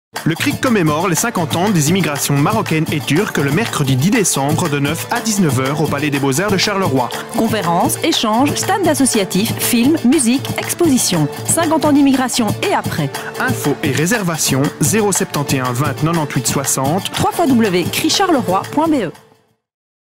50 ans d'immigration, et après?
Le mercredi 10 décembre 2014, le CRIC vous invite à son colloque des Commémorations des 50 ans des immigrations marocaine et turque, après les hommages et célébrations qui ont marqué l’année 2014.

50 ans d’immigration, et après ?
Le mercredi 10 décembre 2014
De 9h à 18h
Au Palais des Beaux-Arts,
Salle des Congrès, Charleroi

Conférence, échanges, stands associatifs, film, musique live, expositions

Avec Anne MORELLI Nouria OUALI Henri GOLDMAN Michel VANDERKAM Hassan BOUSETTA… et bien d’autres encore !

Avec la participation de l’illustrateur Nicolas VADOT
Avec les interventions clownesques de DAISY CROQUETTE

▬▬▬▬▬ QUI SOMMES-NOUS ? ▬▬▬▬▬
Créé en 1997, le CRIC est une ASBL avec comme objectif l’intégration des personnes étrangères.